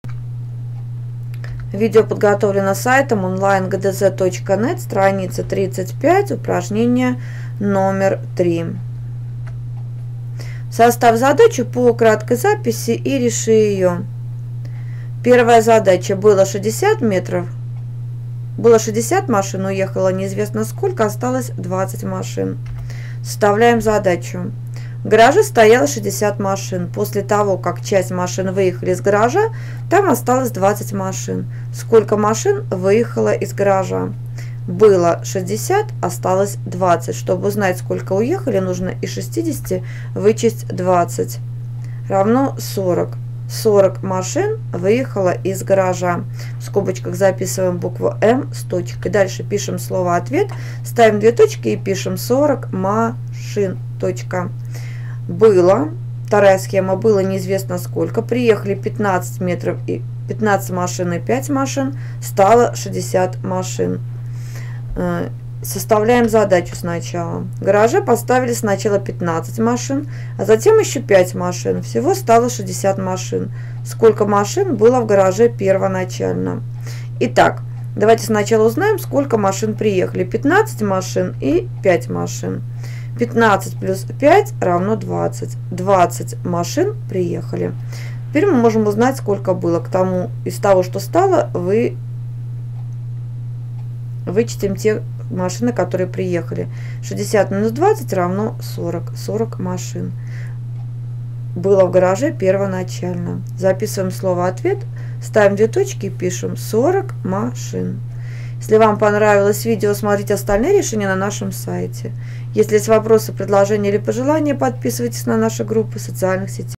Страница 35 Задание 3 – Математика 2 класс (Моро) Часть 2
Для того чтобы выполнить упражнение №3 на странице 35 необходимо уметь составлять задачи по кратким записям, решать простые задачи в 1 действие и составные задачи в 2 действие. Посмотрите наше видео, чтобы всё стало понятно.